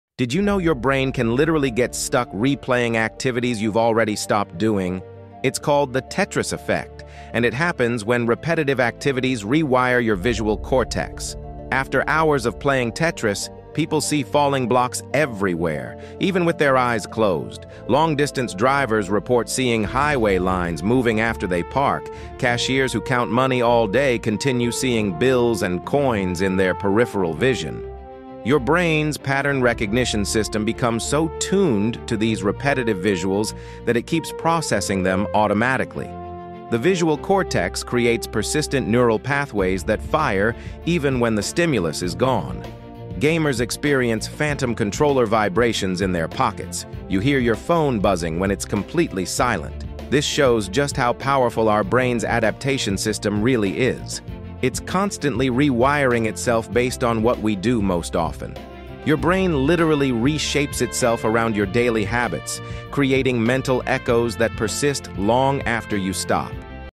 Your Brain Gets STUCK Playing Tetris (The Tetris Effect Explained)
Your brain literally replays games after you stop playing